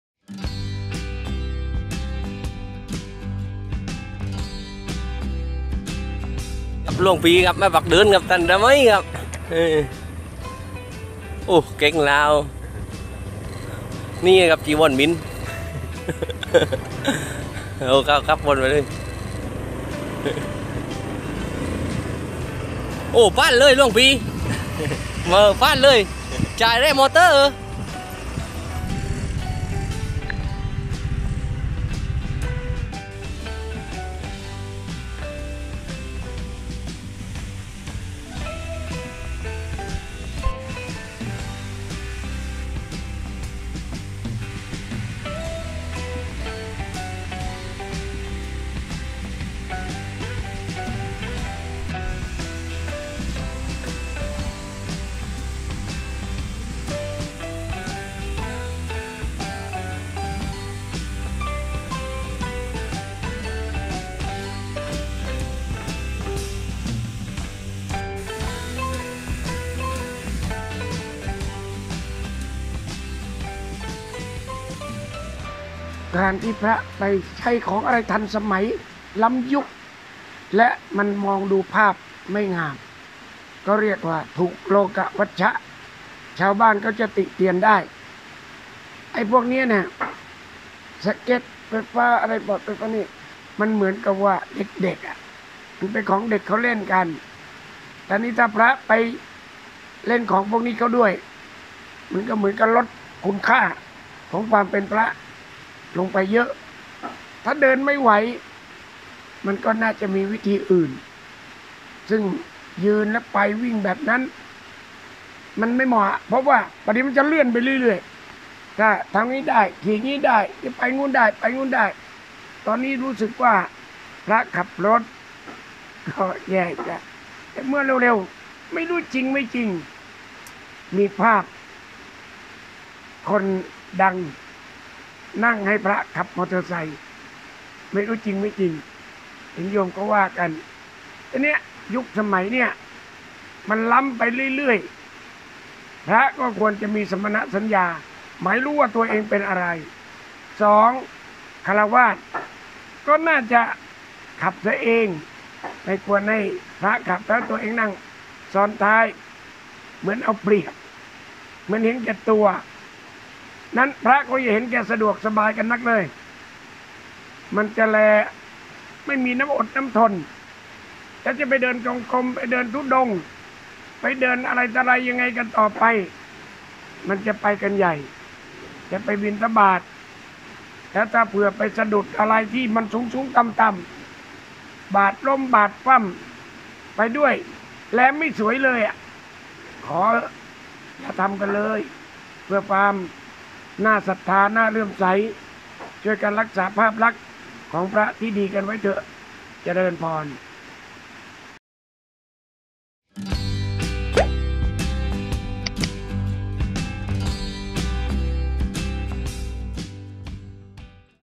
พระพยอม จวก ‘พระสเก็ตบอร์ดไฟฟ้า’ ขอเถอะไม่เหมาะสม
พระพยอม จวก ‘พระสเก็ตบอร์ดไฟฟ้า’ ขอเถอะไม่เหมาะสม วอนรักษาภาพลักษณ์ ความเลื่อมใส-ศรัทธา